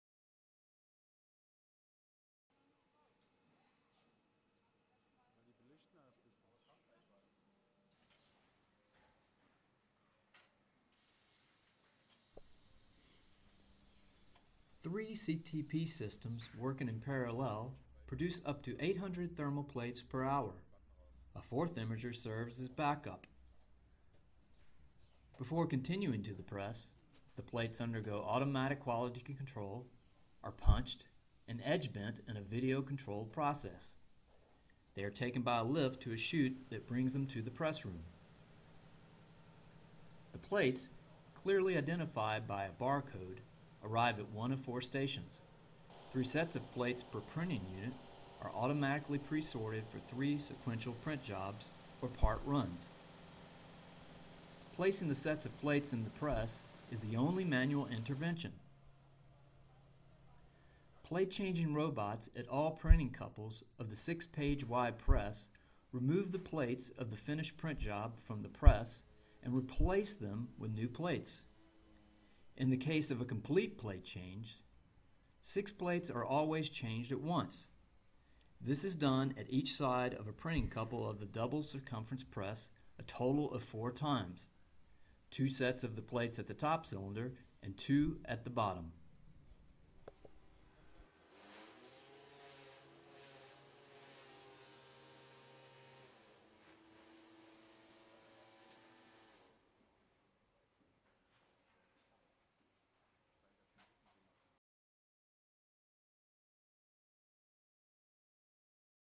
The way of the printing plate from CTP to the press
The way of a printing plate from the computer-to-plate system to the newspaper printing press. The video shows the workflow solution at the Freie Presse in Chemnitz, Germany.